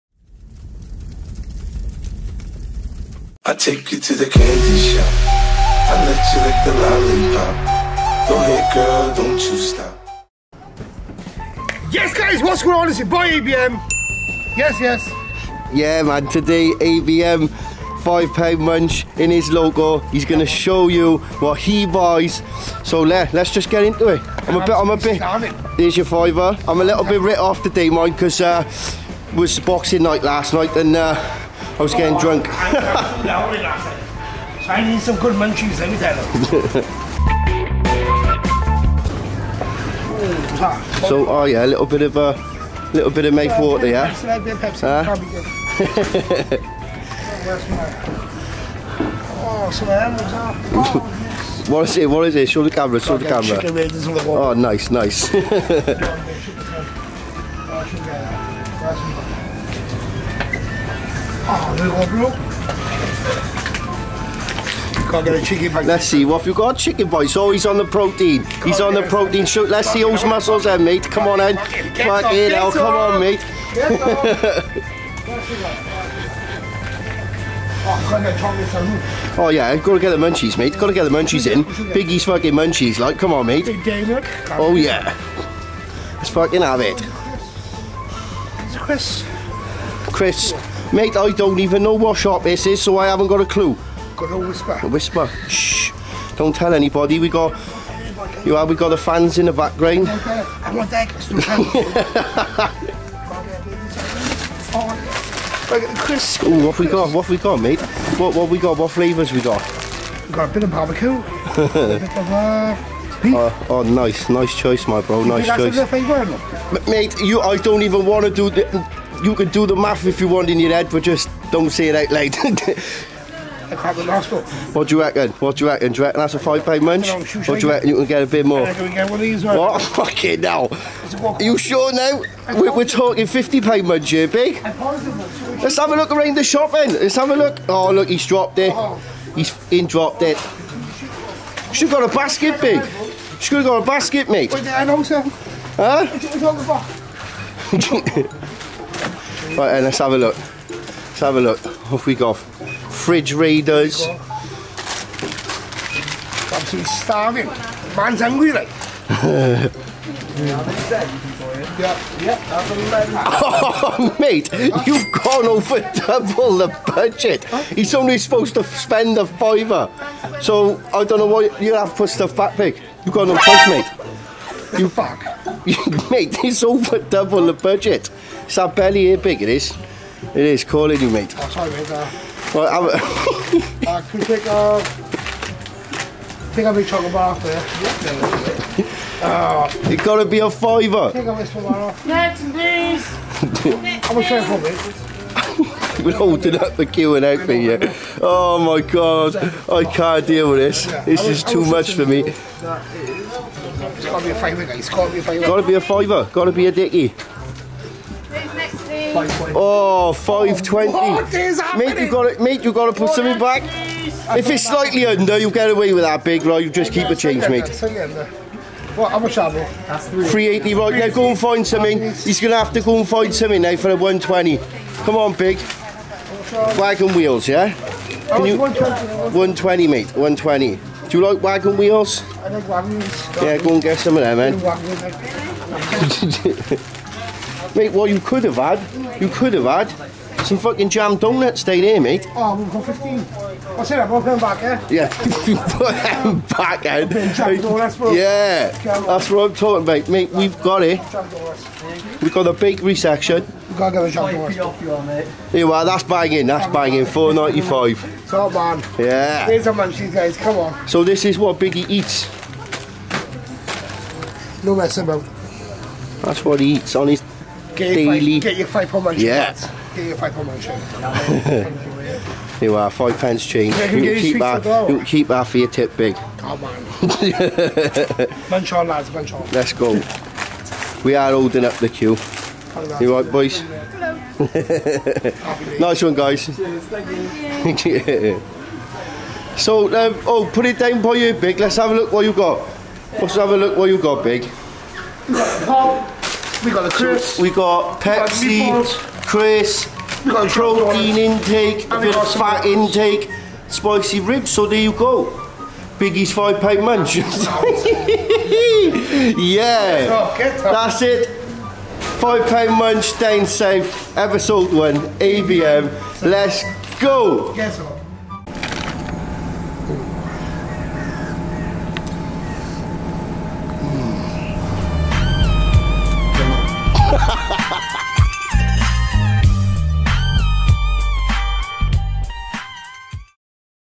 Five Pound Munch Down South - ABM
The wait is over Five Pound Munch Down South is finally here and no better way to start it... First up we have the local legend ABM "Andrew Biggie Morris" Comedian, Musician and just an overall internet famous face be sure to check his content if you havent already on tiktok and other platforms..